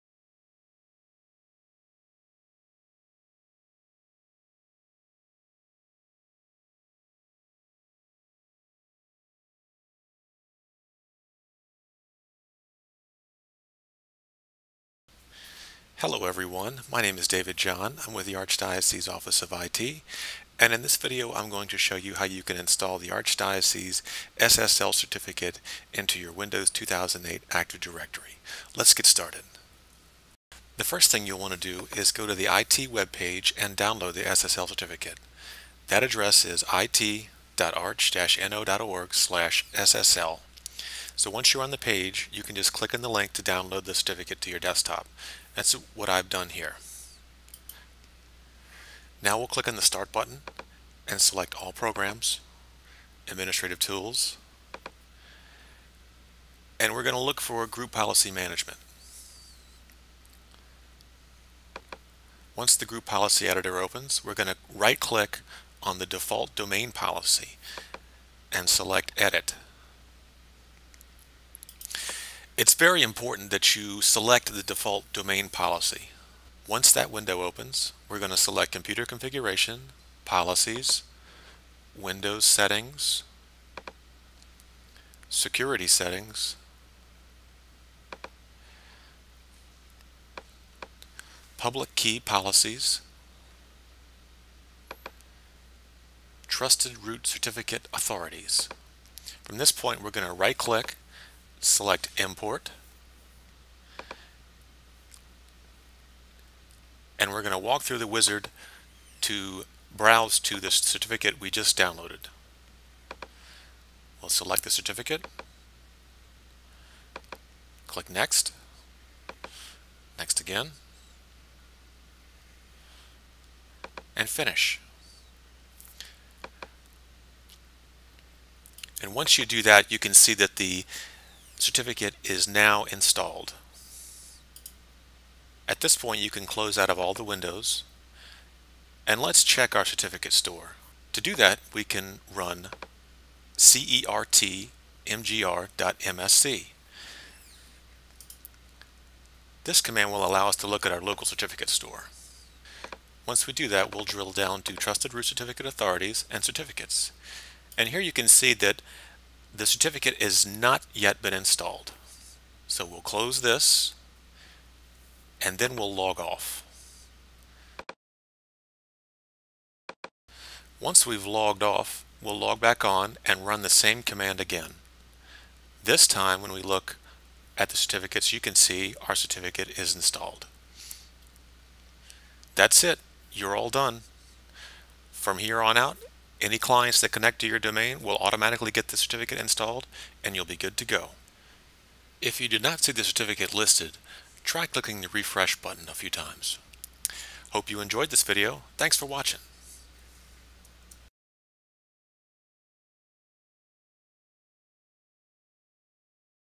Windows 2008 Active Directory SSL Certificate Deployment
A screencast on how to deploy the Archdiocese SSL certificate on Windows 2008